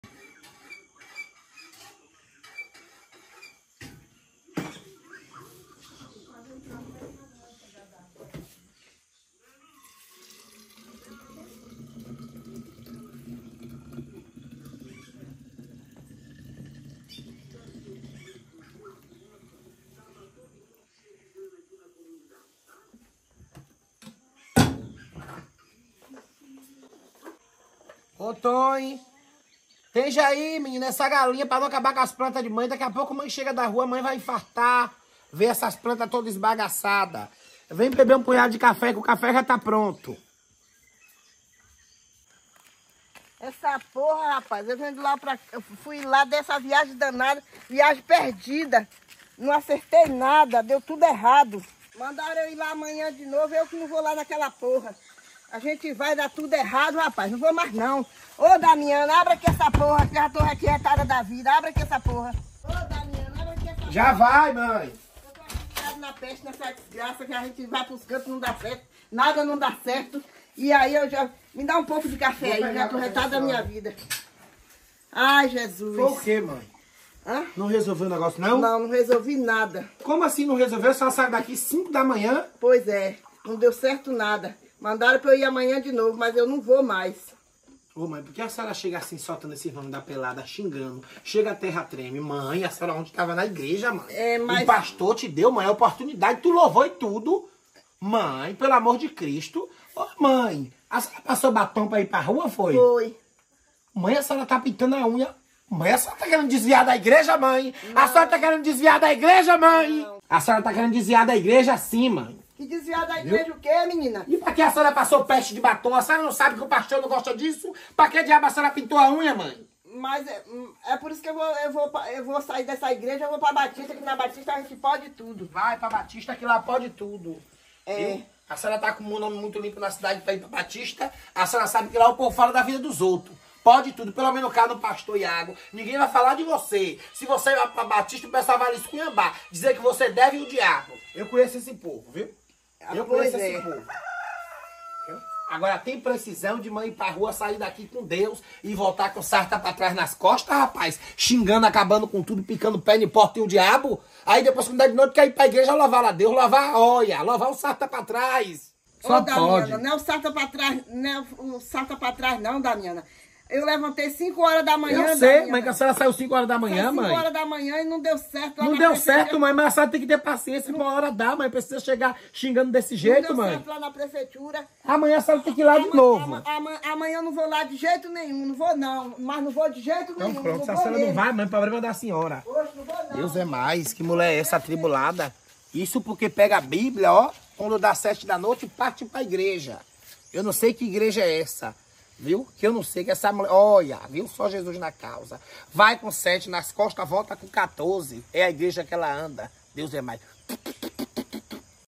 Algo deu errado minha mãe
A Mãe volta toda arrumada da rua. O que será que ela ta aprontando?